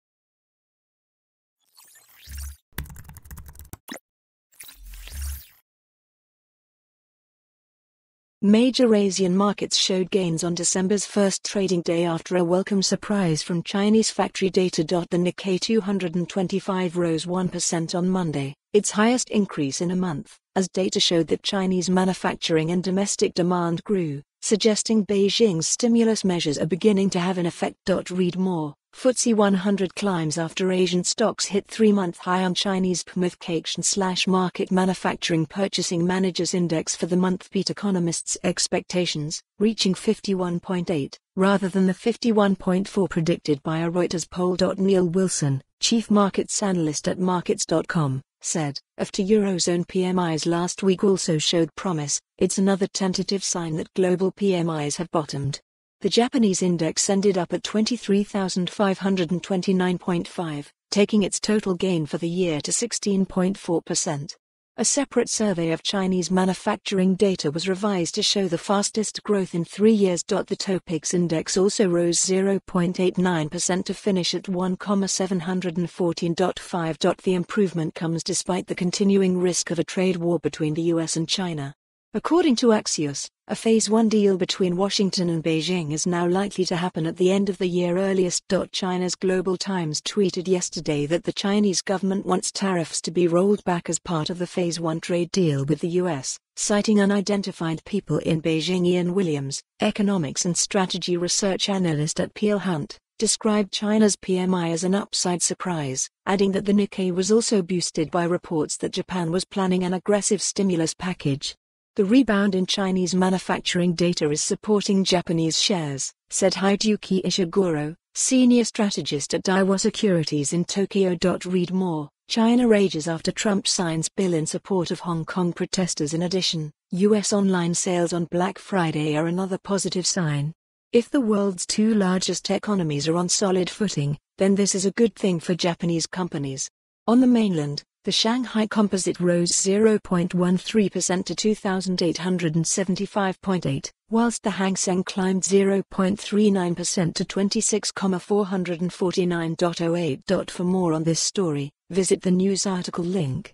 Asian stocks climb as Chinese manufacturing PMI beats expectations
Major Asian markets showed gains on December's first trading day after a welcome surprise from Chinese factory data.The Nikkei 225 rose one per cent on Monday, its highest increase in a month, as data showed that Chinese manufacturing and domestic demand grew, suggesting Beijing's stimulus measures are beginning to have an effect.Read more: FTSE 100 climbs after Asian stocks hit three month high on Chinese PMIThe Caixin/Markit manufacturing Purchasing Manager's Index for the month beat economists' expectations, reaching 51.8, rather than the 51.4 predicted by a Reuters poll.Neil Wilson, chief markets analyst at markets.com, said: A"fter Eurozone PMIs last week also showed promise, it's another tentative sign that global PMIs have bottomed."The Japanese index ended up at 23,529.5, taking its total gain for the year to 16.4 per cent. A separate survey of Chinese manufacturing data was revised to show the fastest growth in three years.The Topix index also rose 0.89 per cent to finish at 1,714.5.The improvement comes despite the continuing risk of a trade war between the US and China. According to Axios, a phase-one deal between Washington and Beijing is now likely to happen at the end of the year earliest.China's Global Times tweeted yesterday that the Chinese government wants tariffs to be rolled back as part of the phase one trade deal with the US, citing unidentified people in BeijingIan Williams, economics and strategy research analyst at Peel Hunt, described China's PMI as an "upside surprise", adding that the Nikkei was also boosted by reports that Japan was planning an aggressive stimulus package."The rebound in Chinese manufacturing data is supporting Japanese shares," said Hideyuki Ishiguro, senior strategist at Daiwa Securities in Tokyo.Read more: China rages af